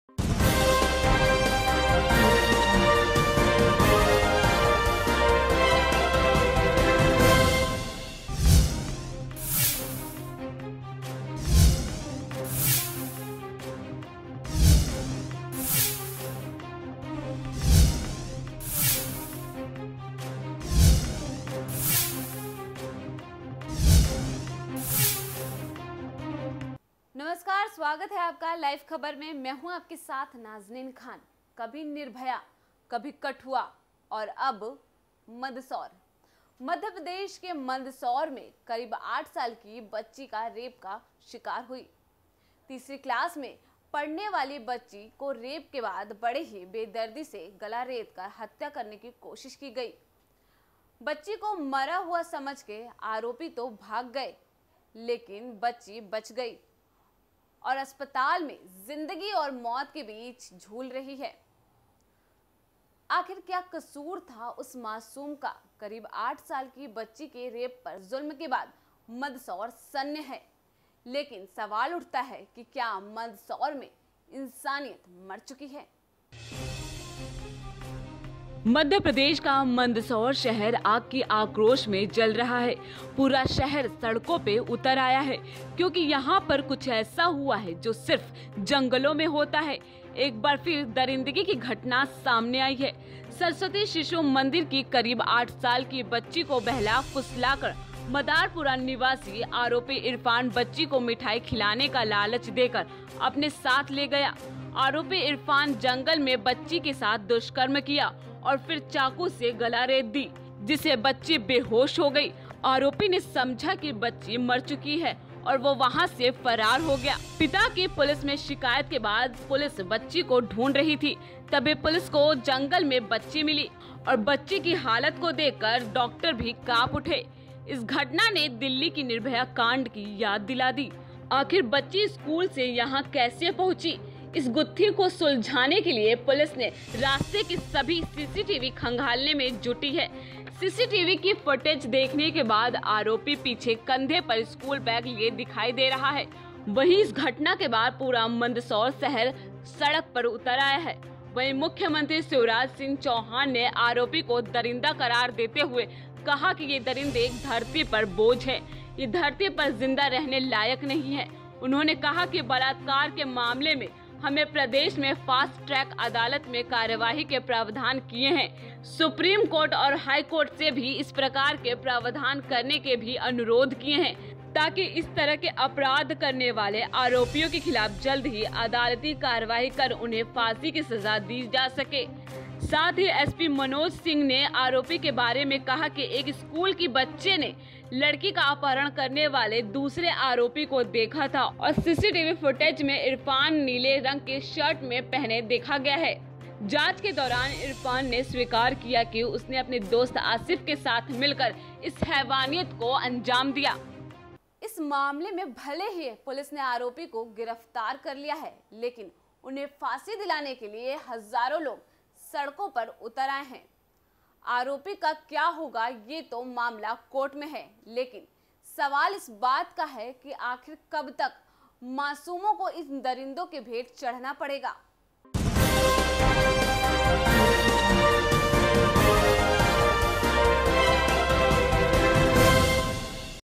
LIVE KHABAR।। मंदसौर में आठ साल की बच्ची से गैंगरेप, 30 JUNE 2018
MANDSOUR GANGRAPE,, 30 JUNE 2018